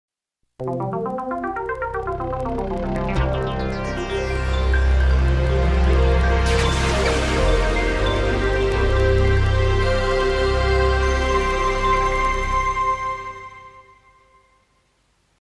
(FAKE) Fox-Columbia TriStar-Warner Home Video (1993-1999)
Seen on VHS, DVD and VCD releases of 20th Century Fox, Columbia Pictures, TriStar Pictures and Warner Bros. films, as well as Lucasfilm productions in Hong Kong, Singapore and Taiwan.